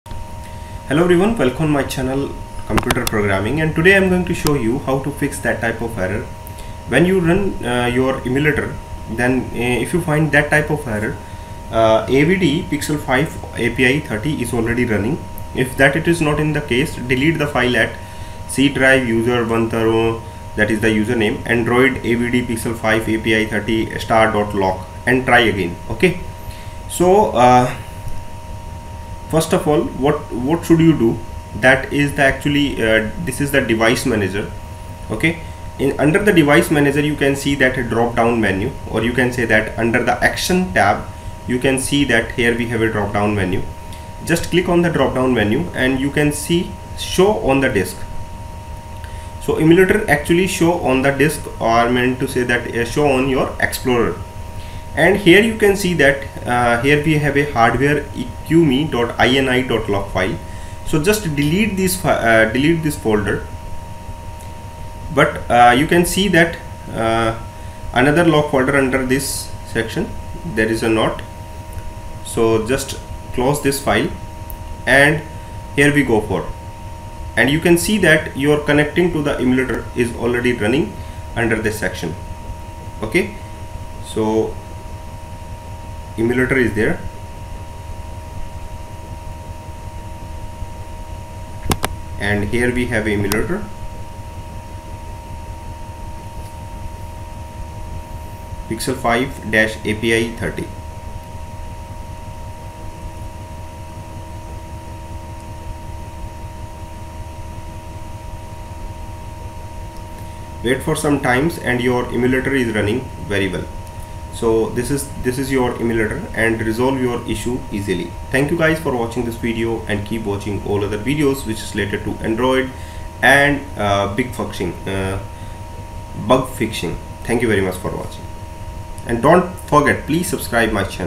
Fix: Android Studio: emulator is already running
When I run my android emulator then i got a error message i.e., AVD emulator-version already running, I try to fix that problem on my level, and I got success.